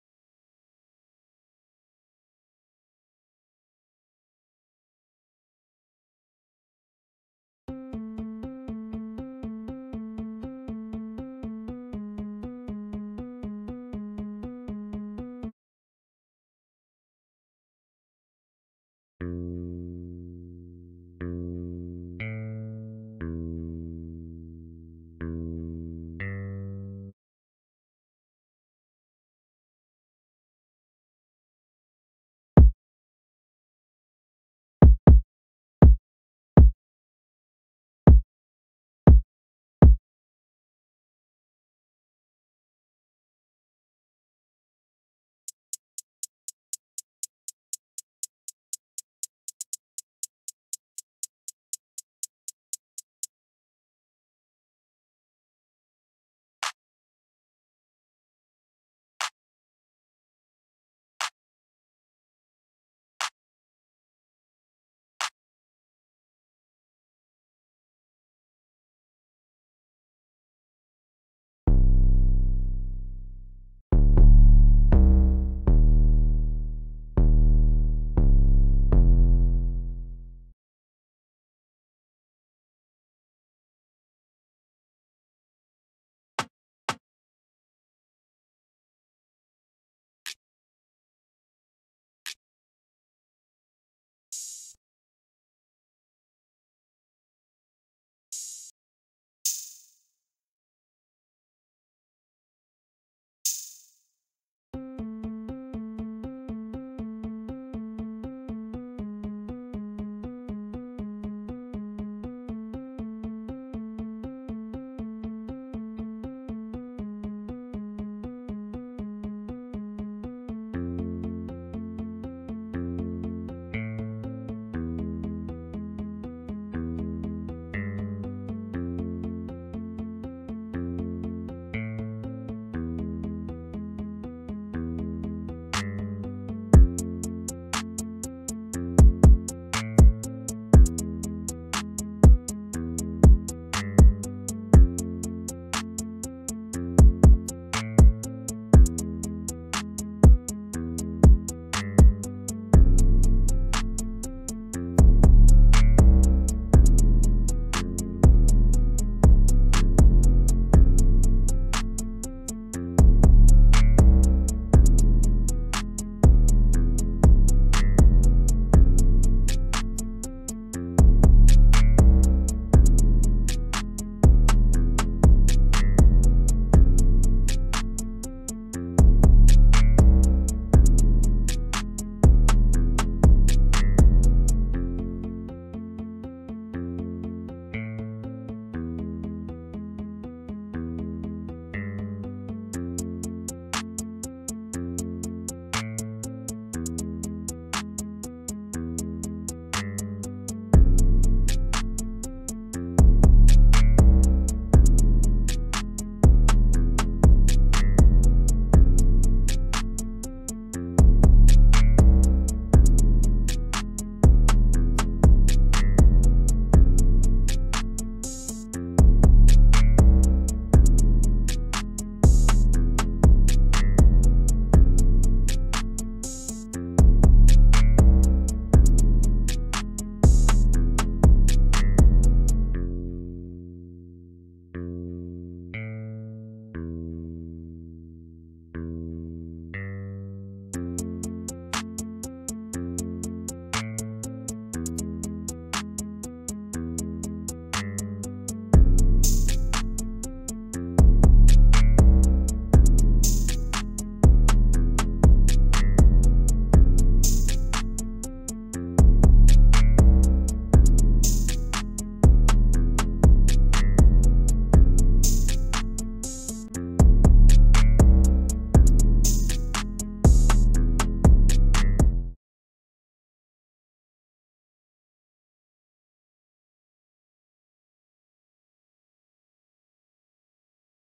Сделал бит в стиле SLAVA MARLOW за 200 рублей!
Я Сделал бит в стиле SLAVA MARLOW за 200 рублей!
Ты можешь купить этот бит и заказывать биты у меня в телеграме!